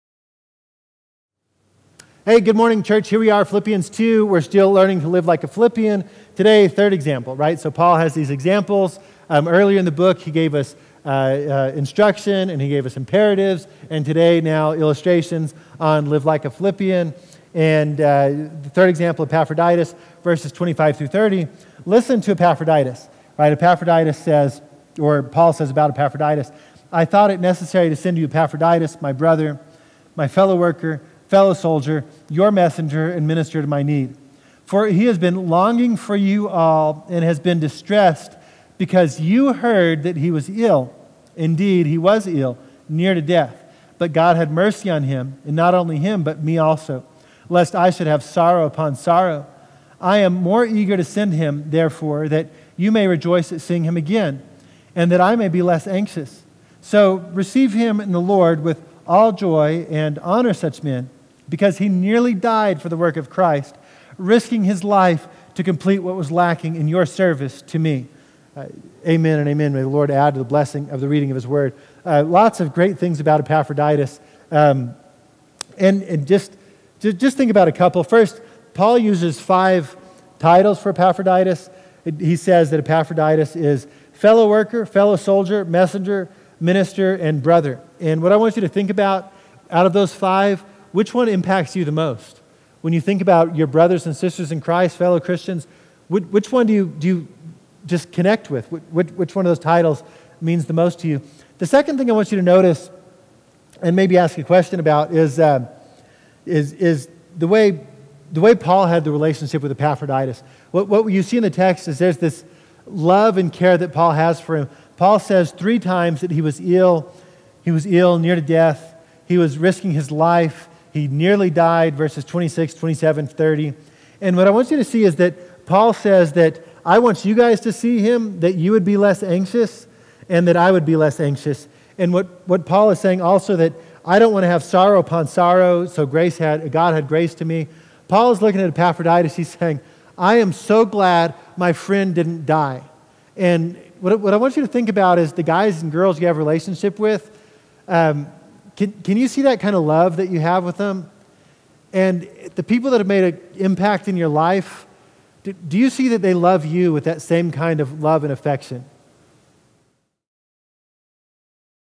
Philippians 2:25-30, The Example of Epaphroditus
Paul talks about Epaphroditus describing the kind of sacrificial love he has for the church. Paul uses five titles for Epaphroditus: brother, fellow worker, fellow soldier, messenger, and minister. Paul cares so much about this brother he wants him to make a full recovery and live. Paul writes as though they have a very close relationship.

Questions:
1. Which of the 5 titles that Paul gives Epaphroditus connects with you best? Why?
2. When you think about Christian brothers/sisters, do you see the kind of love/care Paul had for Epaphroditus?

Philippians 2:25-30